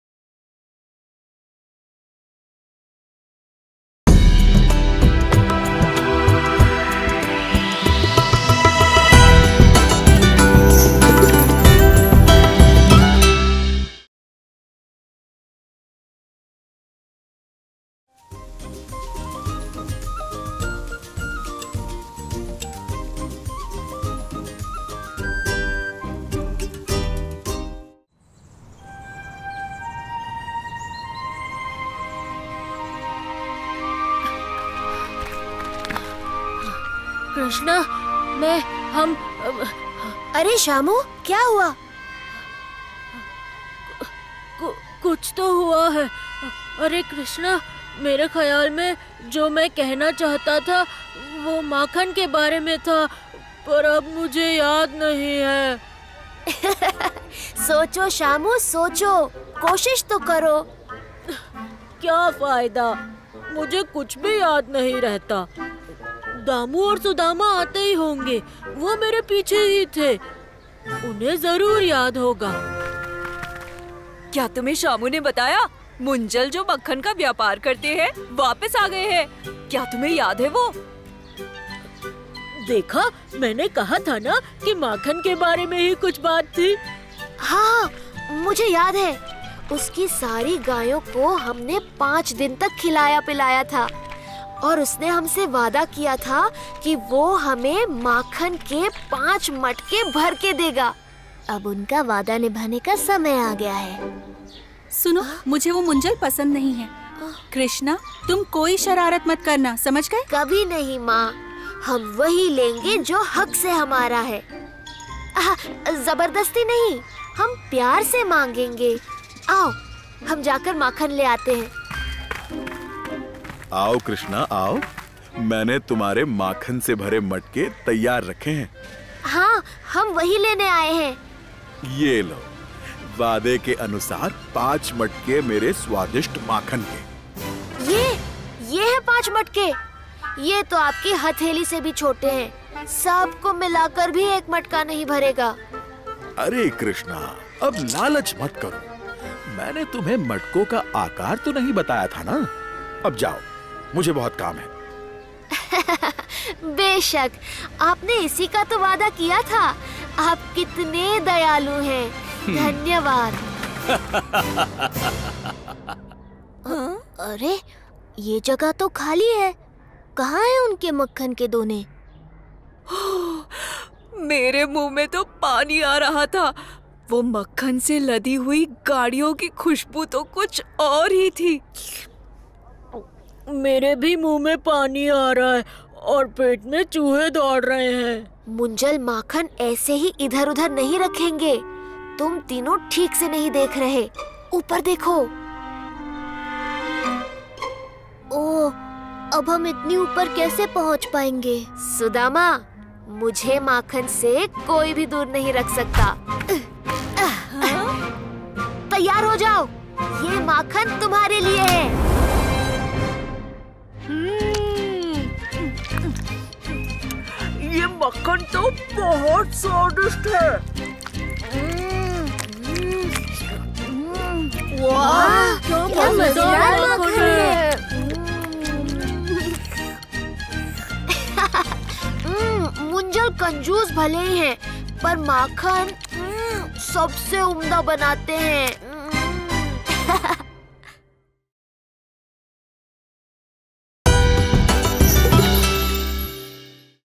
Lord Krishna Stories in Hindi - 05 | कृष्णा की हिंदी कहानियाँ
Mythological Movie present a new series of Krishna.  Krishna is one of the most widely revered and most famous of all Indian divinities, worshipped as the eighth incarnation of the Hindu. In this series, you see the adventurous life of Krishna.